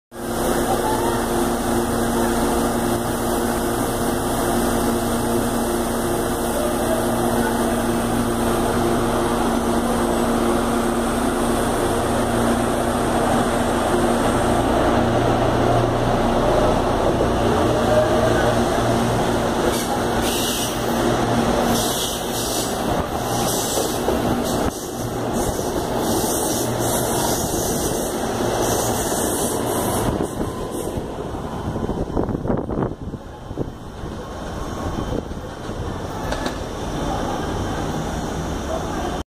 CPTM - TUE Série 7000 Partindo da Estação Pirituba - Linha 7 Rubi
TUE CAF/Mitsubishi Série 7000 (Q096) Partindo da Estação Pirituba Sentido Francisco Morato.

Obs: Restam 2 Séries (Q096 e Q028) da linha 7 que em breve vai ser transferido para Linha 9 - Esmeralda , que a série 7500 vai se transferir para Linha 10 turquesa em Testes dessa linha.